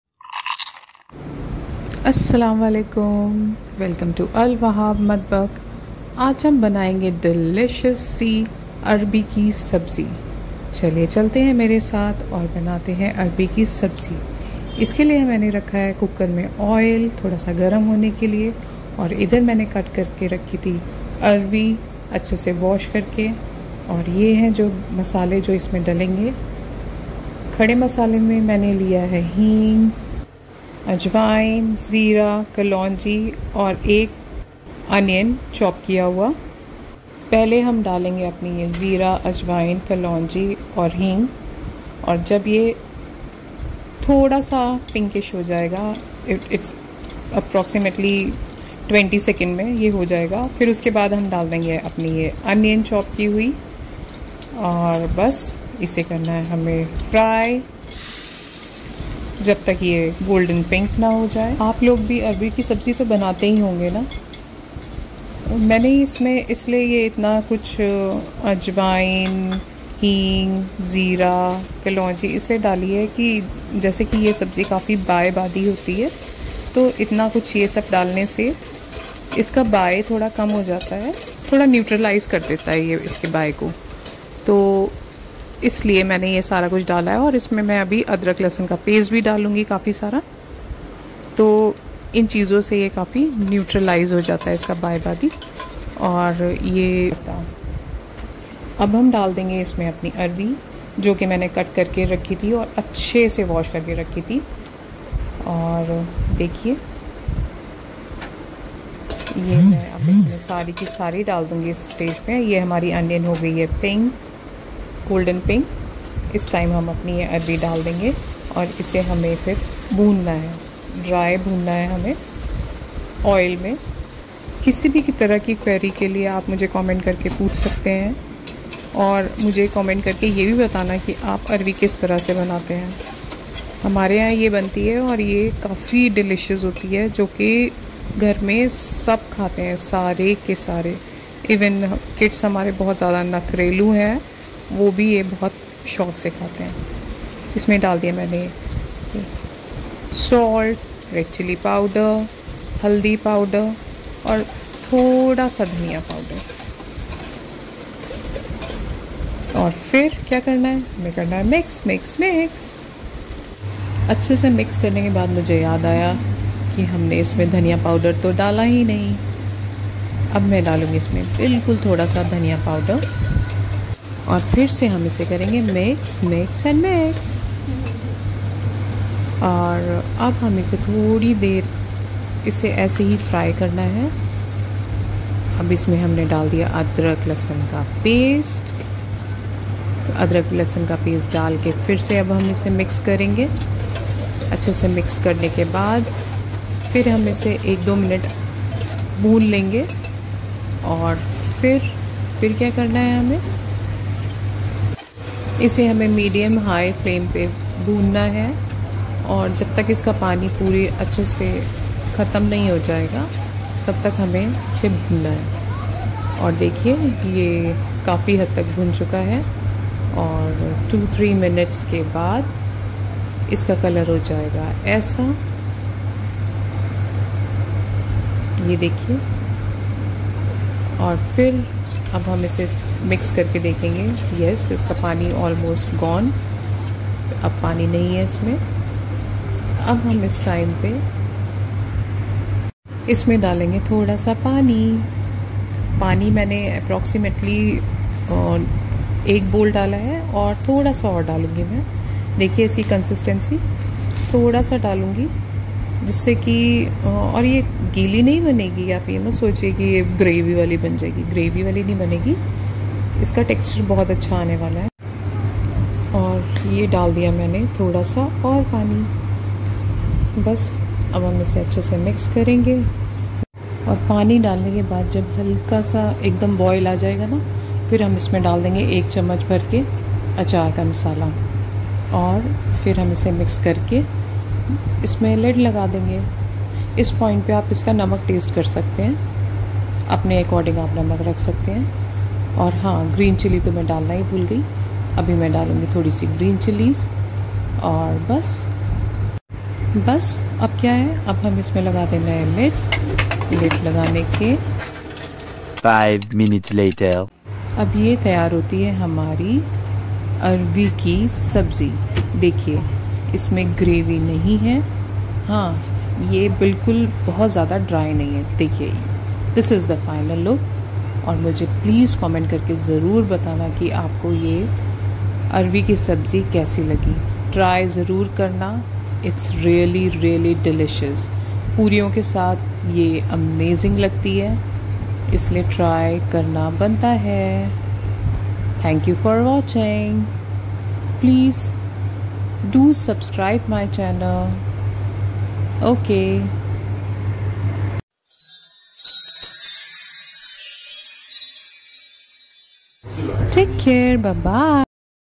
Tasty Arbi Ki Sabzi 😋 Must Try
Assalamoalekumm 🧡
Ajki Special recipe hai
Arbi ki Sabzi 😋 Must Try
Aloo Poori ka Combination to bohot suna hoga
Leki agar Arbi aur poiri ka Combination bana lia to Aloo poori ke Combination ko bhool jaoge
 Agar ek bar aese Arbi ki Sabzi bana li to
Arbi apki Favorite ban jaegi
Ye Asan aur Tasty Recipe hai Jise try karna must hai easy bhi hai aur Purio ke saath Aloo ke Combination ko ache se beat karti hai
Agar Apne poorio ke saath ye try karli to aloo poori ke combination ko bhool jaoge
Try zaroor karna tabhi pata chalega ki ye kitni yummy hai 😋😋😋
Kisi bhi query ke lie ap mujhe comment karke pooch sakte hai